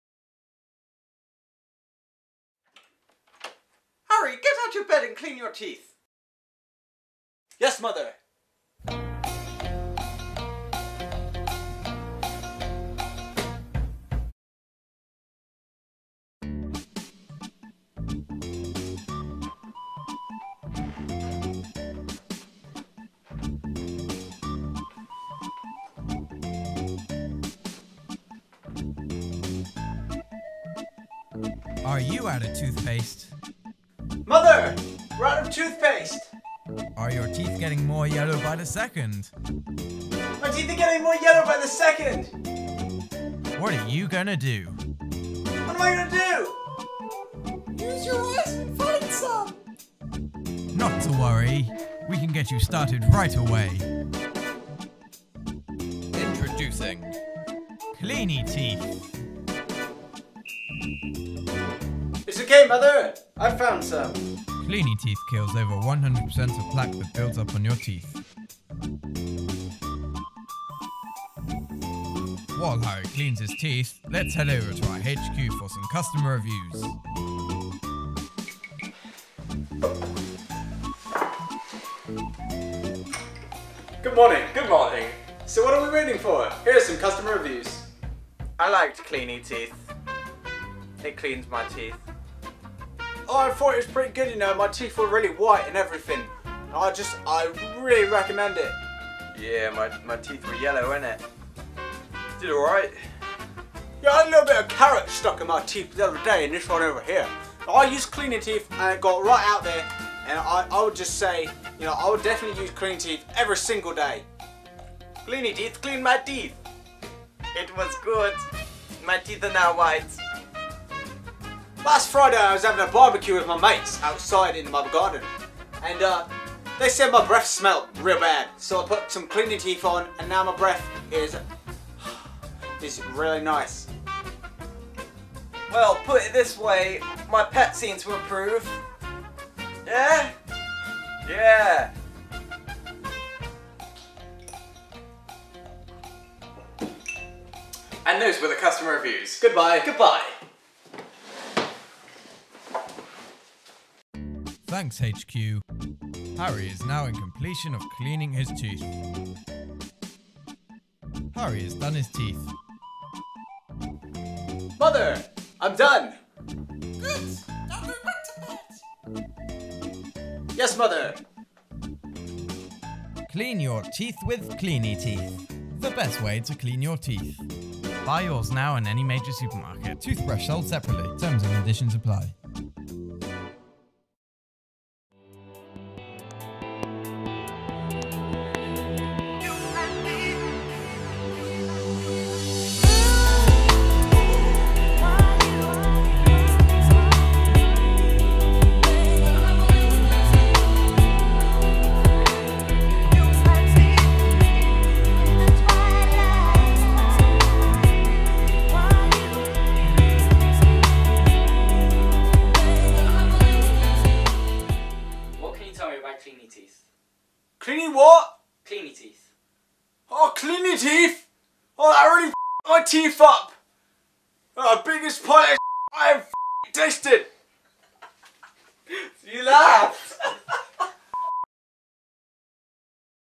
Bad Commercials - KleenyTeeth | Luke and Harri
Luke and Harri discovers a brand new Bad Commercial under the name of Kleeny Teeth

Comment/ Like/ Share/ Subsribe